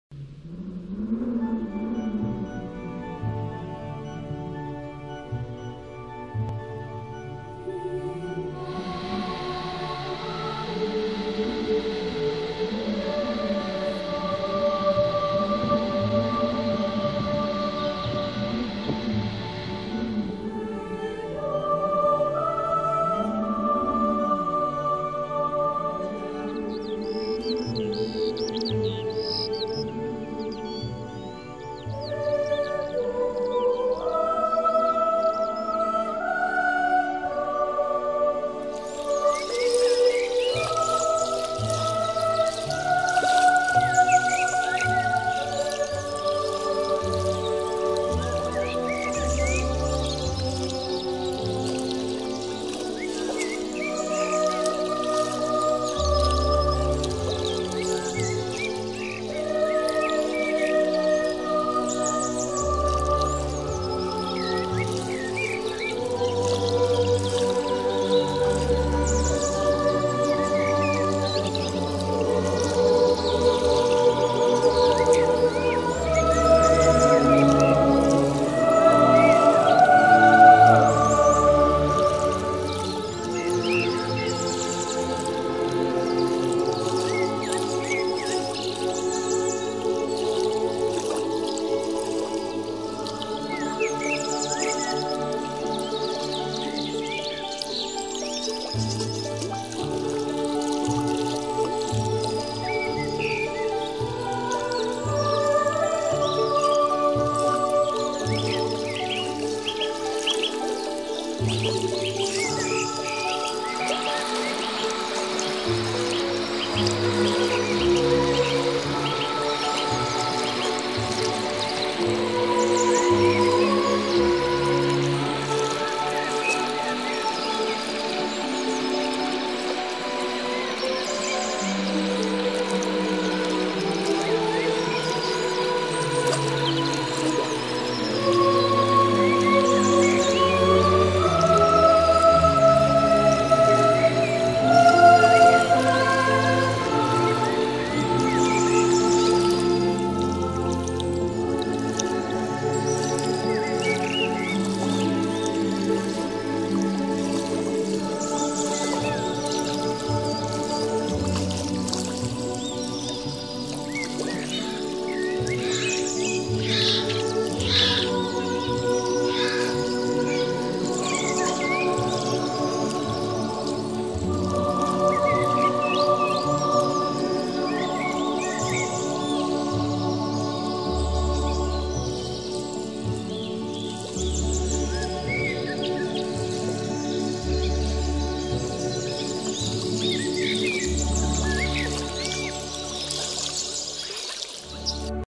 balade hivernal à Moutier en puisaye
des sentiers le long du Loing
autour de l'étang